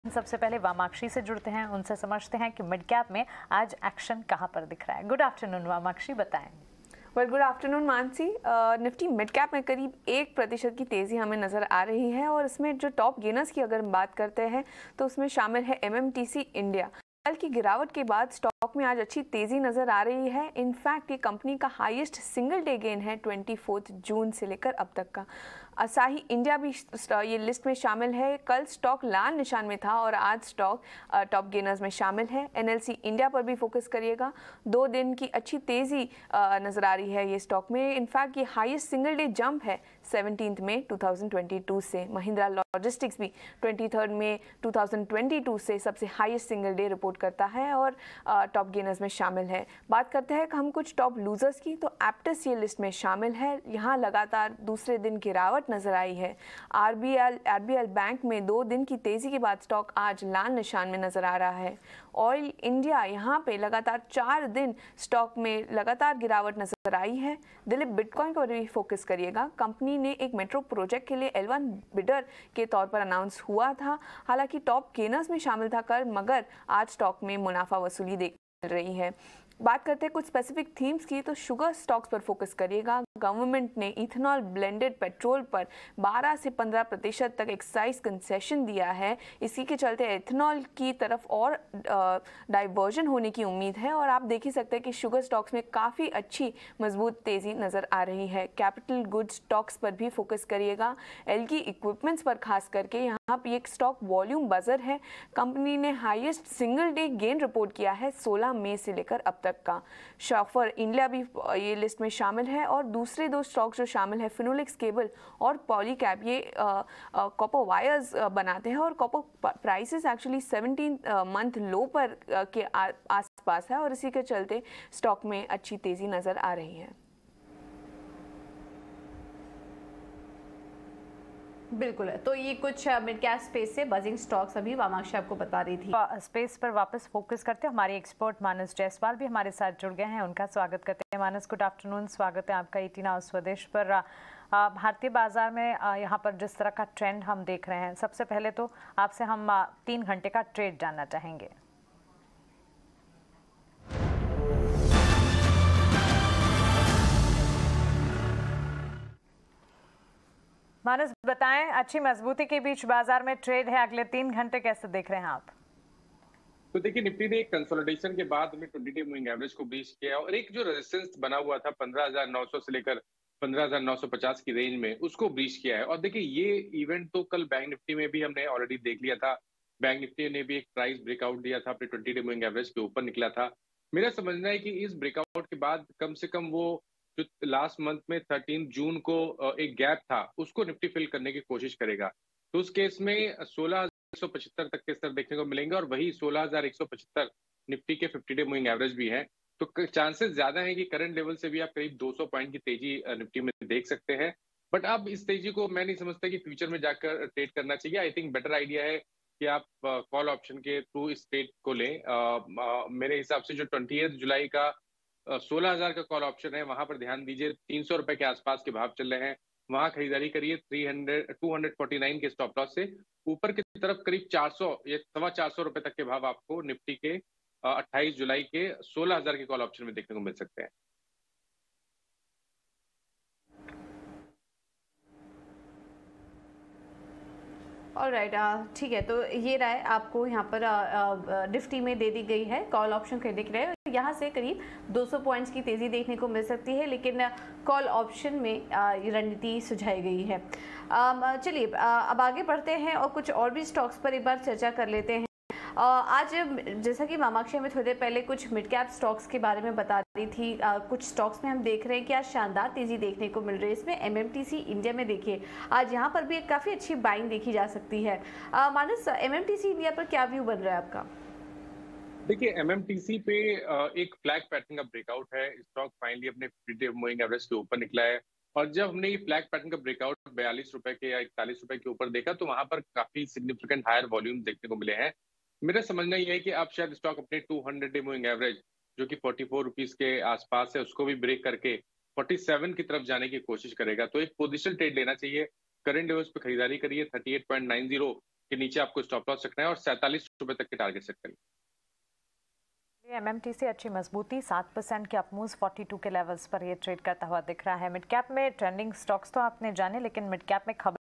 Best 5 Midcap Stocks: इन Midcap Stock में पैसे लगाने से मिलेगा शानदार Return | Share Market News
Midcap Stocks Outlook | Nifty Midcap Today | ETNow Swadesh Live |
Nifty Midcap में आज कौन से Stocks में है बढ़त?
जानिए Midcap Space में Top Stocks To Trade